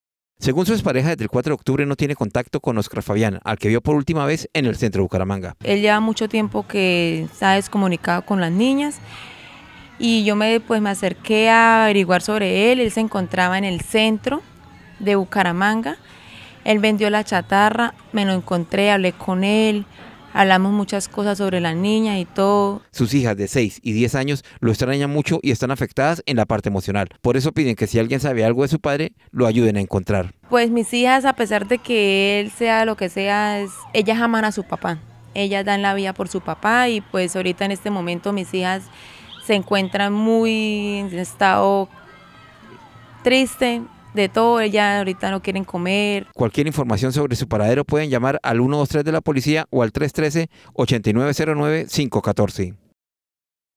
Un hombre desapareció del centro de Bucaramanga hace tres semanas | Oro Noticias
Un hombre desapareció del centro de Bucaramanga hace tres semanas. Su expareja y sus hijas lo buscan.